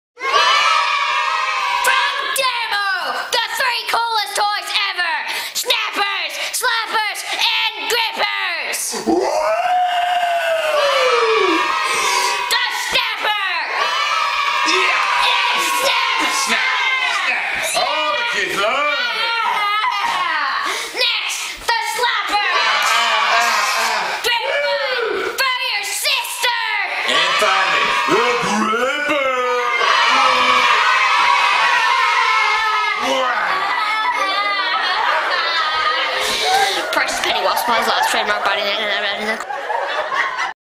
DAMMO Toys' new Snapper! Slapper! and Gripper! toys!
Introducing the THREE latest toys from DAMMO!